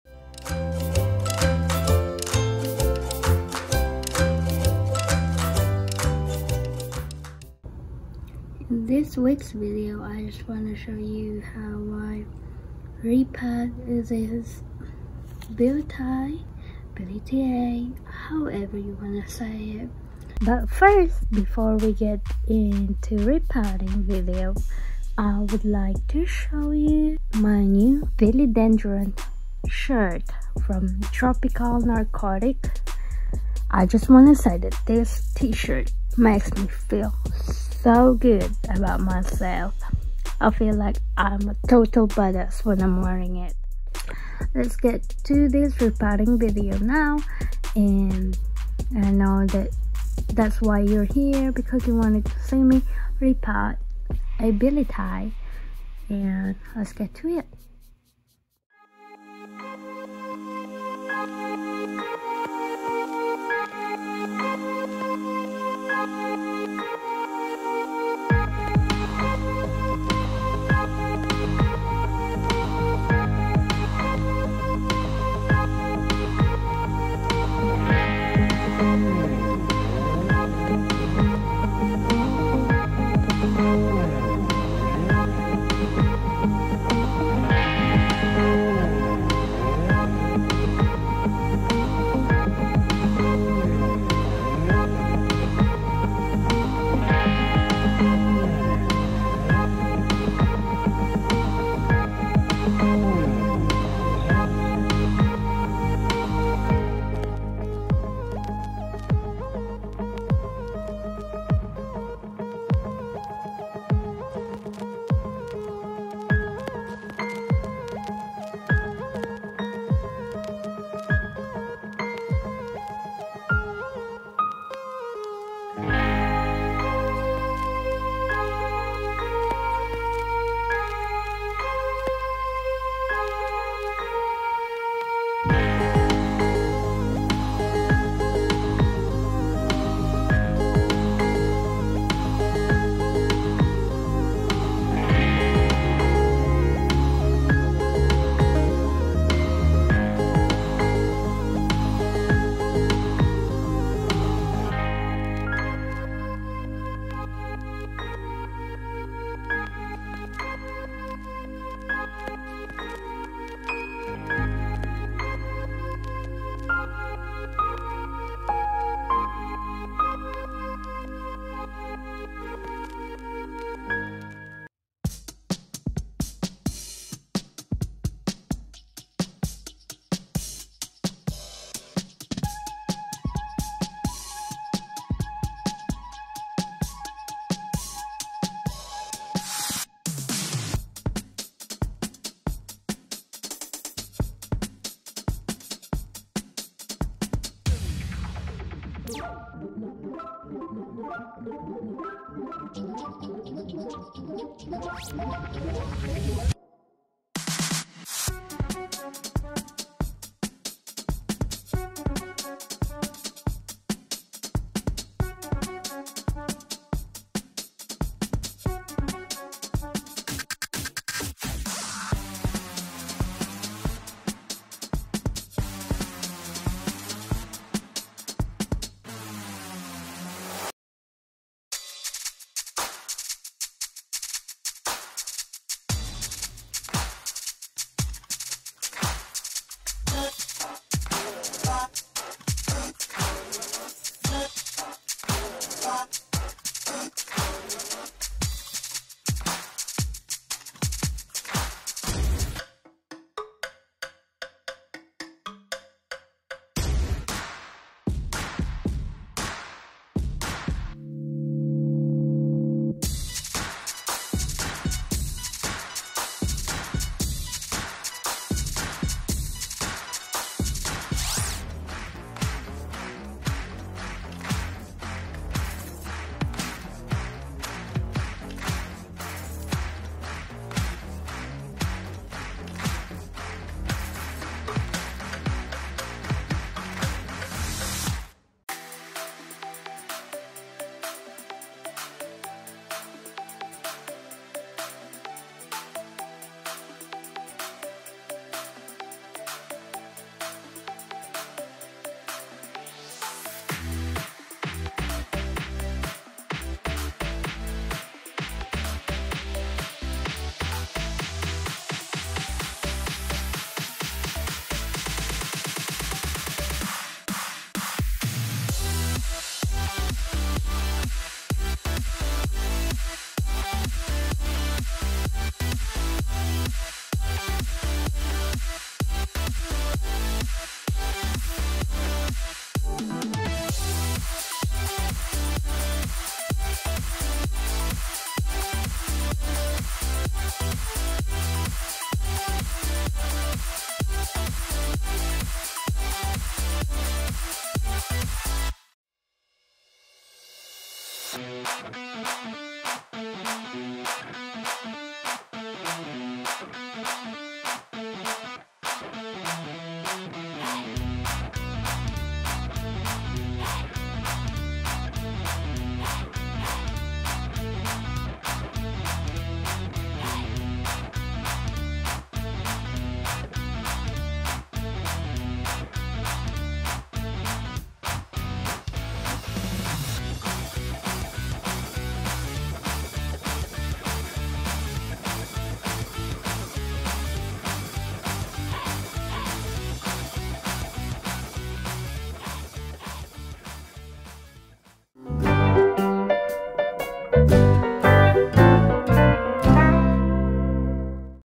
I FEEL LIKE A BAD—- REPOTTING BILLIE! | PHILODENDRON BILLIETIAE
Hey! It’s officially spring season so it’s a perfect time to upload this repotting video this week! I have been putting this plant chore for so long. I was told not to repot it just because the root was trying to escape from the drainage holes. Thank you internet people! Anyho I no longer wanted to stick to internet rules made up by a bunch of people. I didn’t wait for a year before repotting this bad—- plant I just went ahead and done it. Thankfully the plant is so much happier! Keep in mind losing older leaves are normal. As long as the roots are fine and not rotting. I have got nothing to worry about.

Product used in the video:

White clear pot

Favorite Online Plant Shops